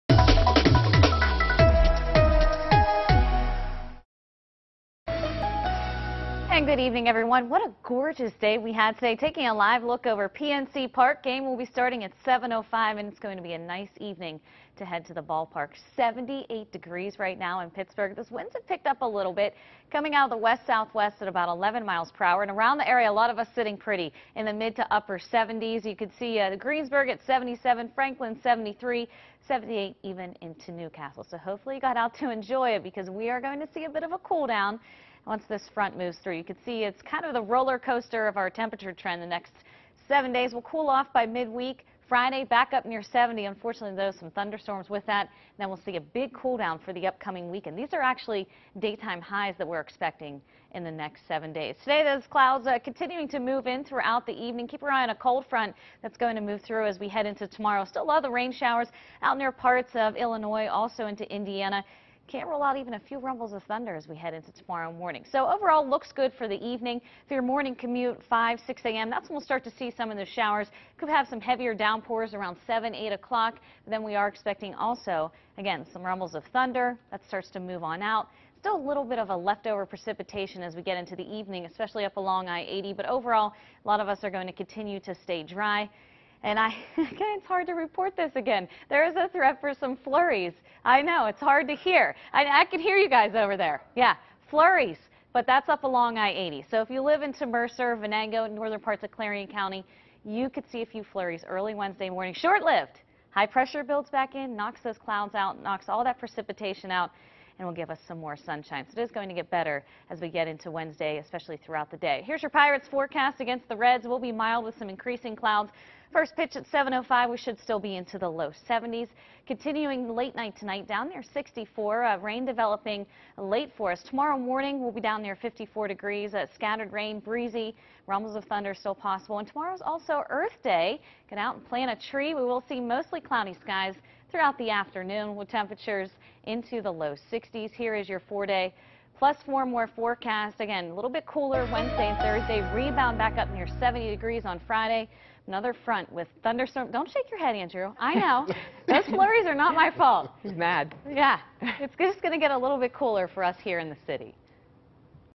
Pittsburgh's Action Weather forecast
Meteorologist Ashley Dougherty has Pittsburgh's Action Weather forecast.